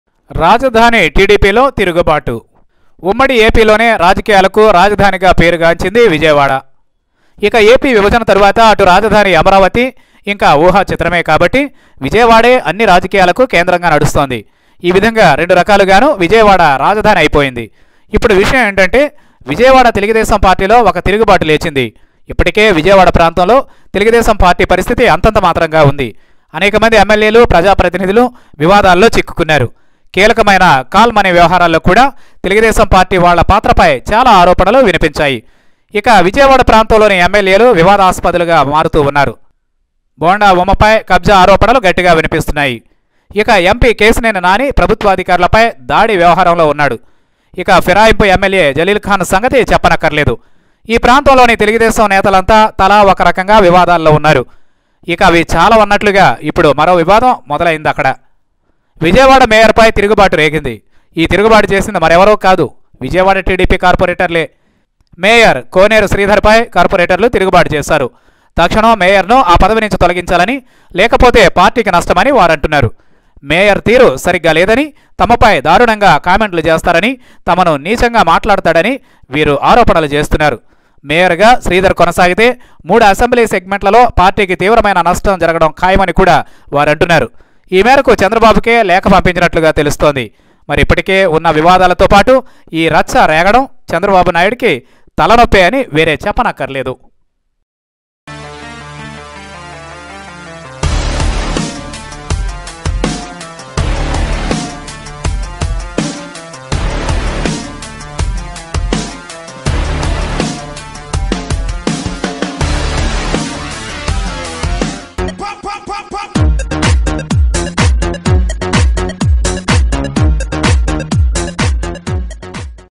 రాజధానిలో రచ్చకెక్కిన తెలుగు తమ్ముళ్ళు| Internal Group War in VIjayawada TDP| Mayor vs MLA
Internal Group War in VIjayawada TDP| Mayor Sridhar vs MLA Gadde